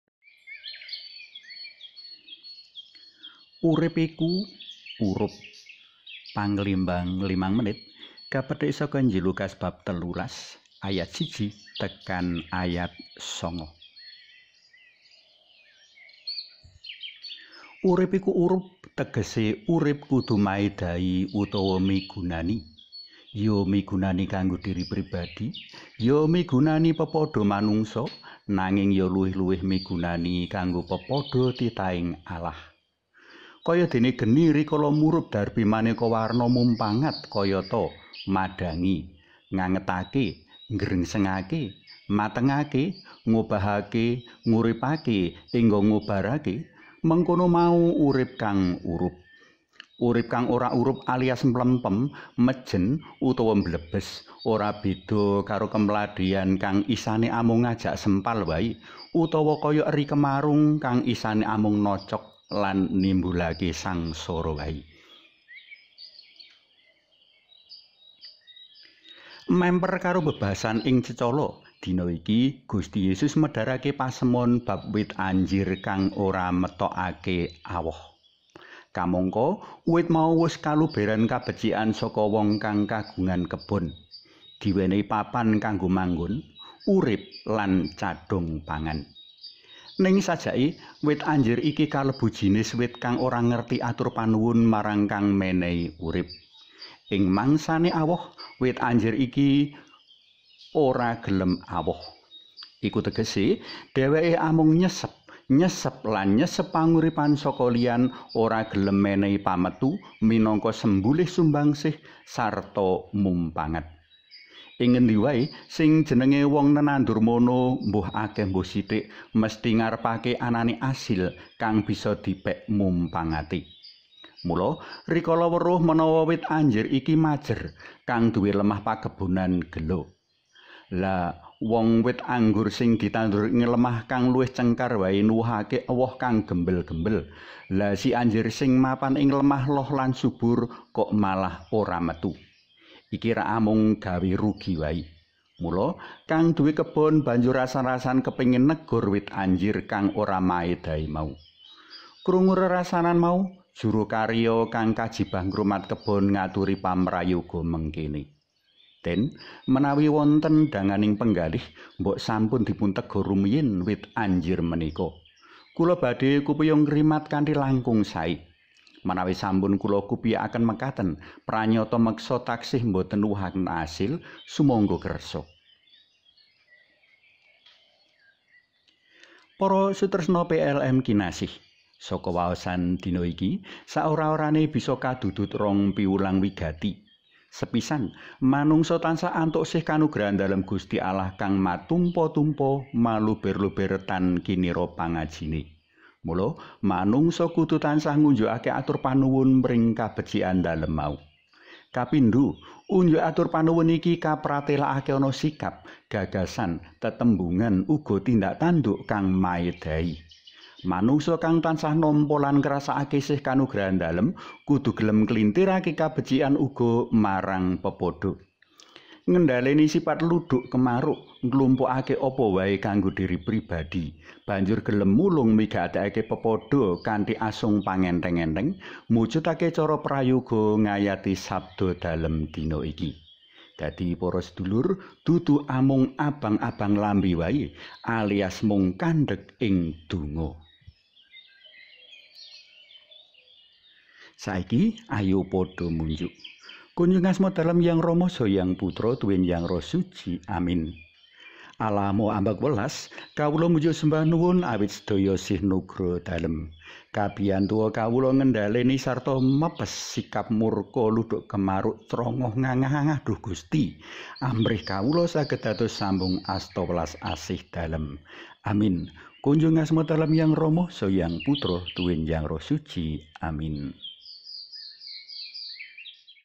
(Audio Renungan) Panglimbang Limang Menit | Br. Antonius Mungsi, O. Carm | 20 Maret 2022
Panglimbang Limang Menit (Renungan Harian berbahasa Jawa) edisi 20 Maret 2022 | "Urip Iku Urup" (Luk. 13:1-9).
Renungan harian Kitab Suci berbahasa Jawa ini dimaksudkan untuk melestarikan budaya Jawa sembari memelihara iman berdasarkan bacaan-bacaan liturgis Gereja Katolik.